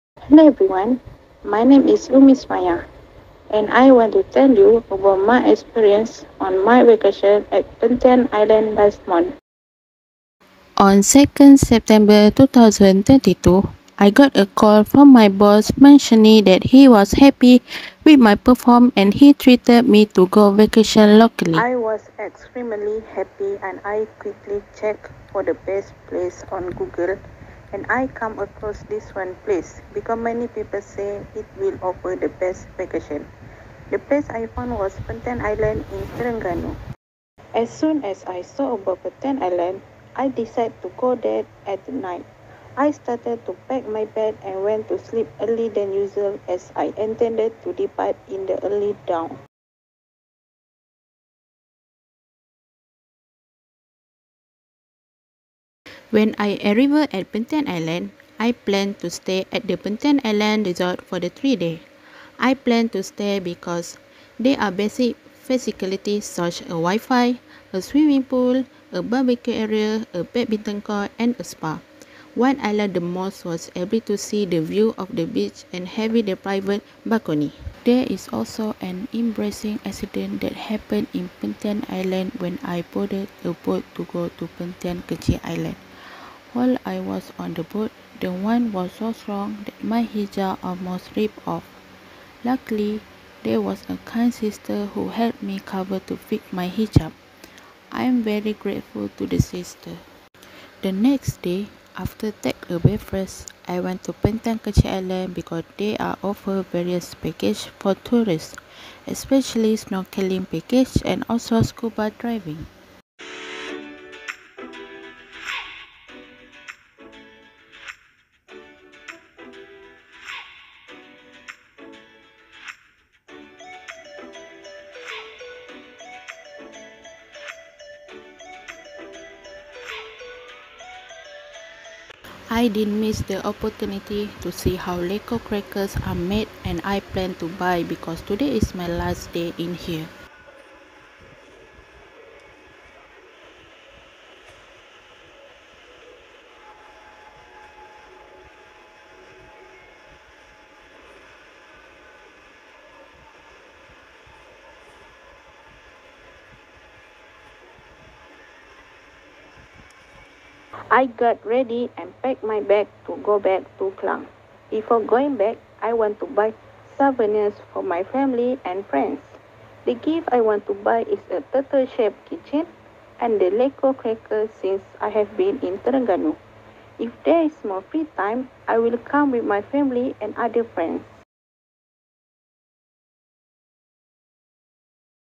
Adventure Of A Lifetime | Perhentian Island | Ummi Summayah
Assalamualaikum wrb and hello everyone.

Have you ever been to Perhentian Island?

Perhentian Island has two main islands, Perhentian Besar("Greater Perhentian") and Perhentian Kecil ("Lesser Perhentian").

If you like scuba-diving, snorkeling and swimming, Perhentian Island is the place to go.

Ummi❤️